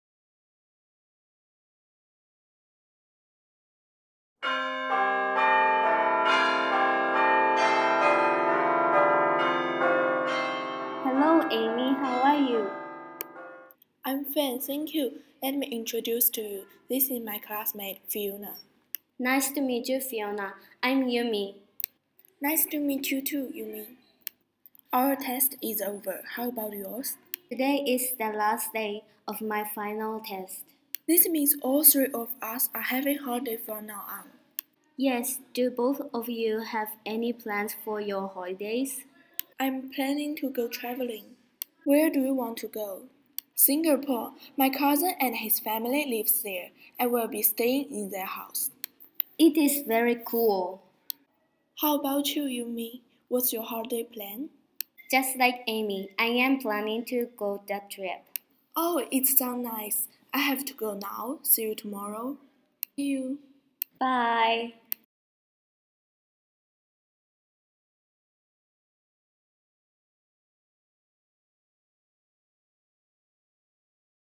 Our Normal Life
Grade 8 and 9 Intensive English Programme students created their own animations as part of a literacy project.